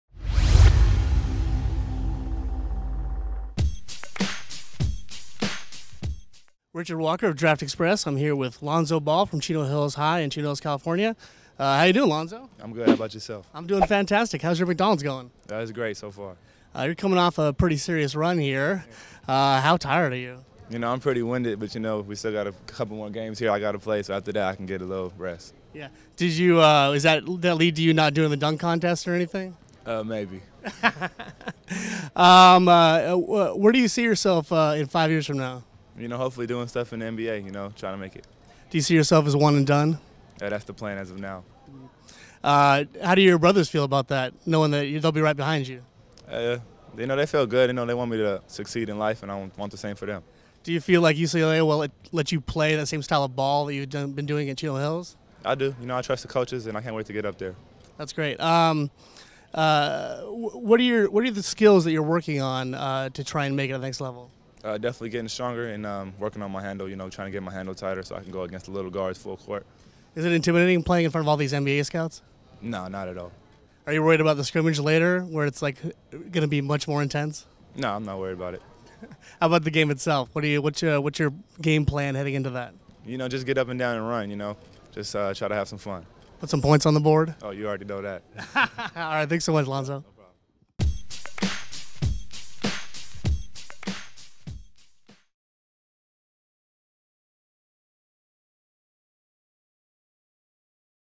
Lonzo Ball - 2016 McDonald's All American Interview
UCLA recruit Lonzo Ball talks with DraftExpress at the 2016 McDonald's All American Game.